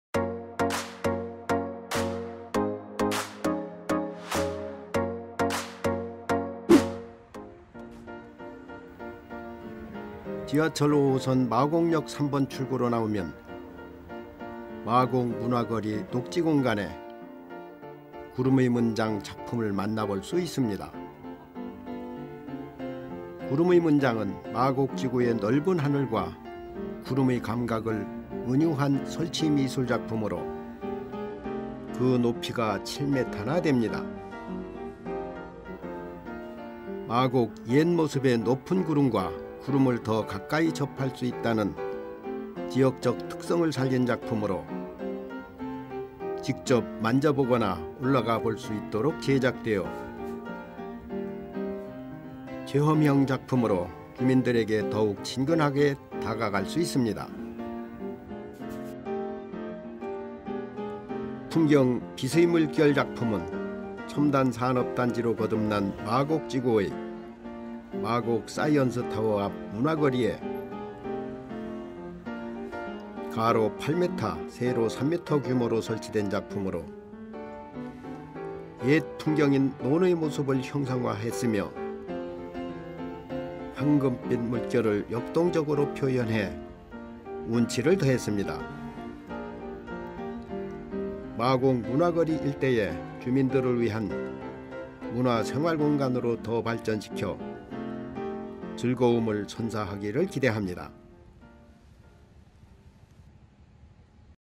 [지금은 강서시대!] 걸어서 동네 한바퀴 [마곡문화의거리 feat.공공미술 프로젝트]
강서구민이 참여하는 강서영상크리에이터
'지금은 강서시대!(김계환)'에서 제작한 영상입니다.

서울시와 문화체육관광부에서 코로나19로 지친 시민들에게 새로운 활력을 불어놓고자
서울시 25개 자치구 전역에 37개의 공공미술품을 설치하였습니다.
그 중에 강서구 마곡지구에 위치한 공공미술품을 소개합니다!
'풍경-빛의 물결'과 '구름의 문장' 2개의 미술품, 같이 구경 가보실까요?